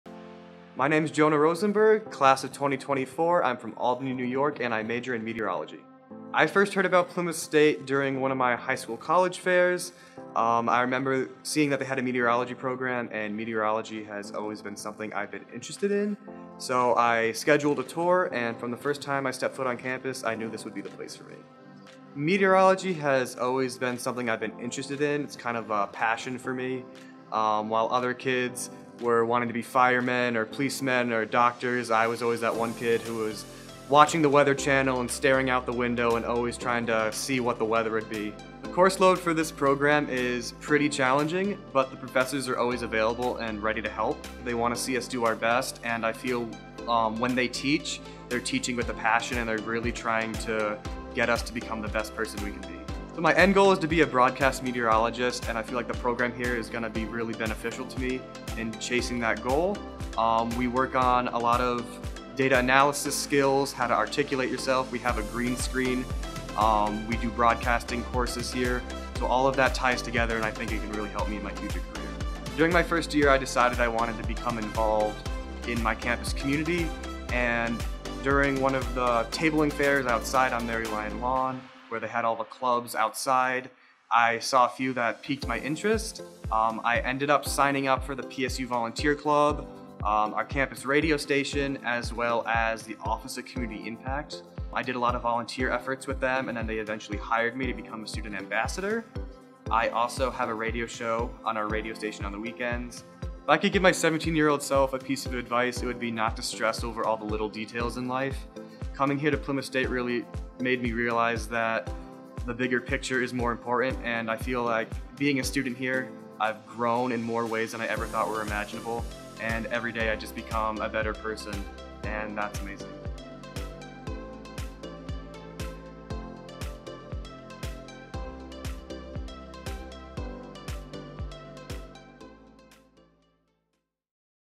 PSU in Two: Jonah Rosenberg '24
Meteorology major, Jonah Rosenberg ’24 from Albany, NY, has always had a fascination with weather. In his second year as a meteorology student, he is well on his way to a fulfilling career with the help of his caring, passionate faculty and his, now, lifelong friends. Take a look.